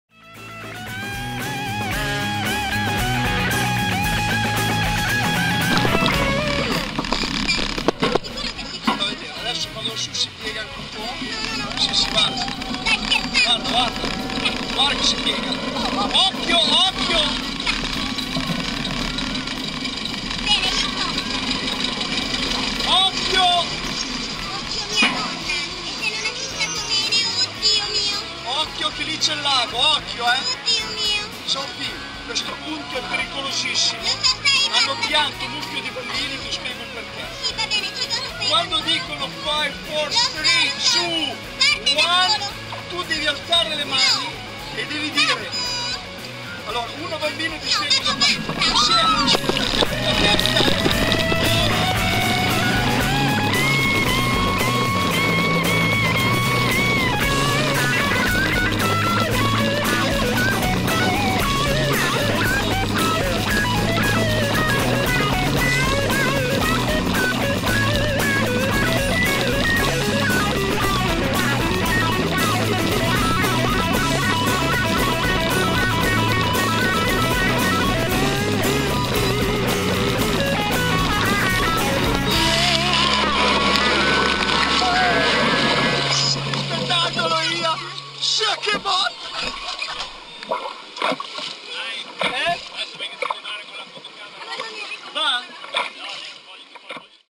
on board Divertical - babbo e figlia davvero imperdibili - POV HD Mirabilandia
Un video divertentissimo girato all'insaputa di una famiglia che affronta Divertical, sual Altezza il Water Coaster più alto del mondo.
Occhio ai dialoghi!
Nota di Mirabilandia
Il video è a loro insaputa poiché era stato detto loro che sul treno era stata posizionata una micro macchina fotografica e che il giro era di prova. Neanche noi ci aspettavamo un video cosi divertente, la telecamera era stata messa in rec per verificare le inquadrature. Per poi decidere dove e come scattare foto e realizzare inquadrature.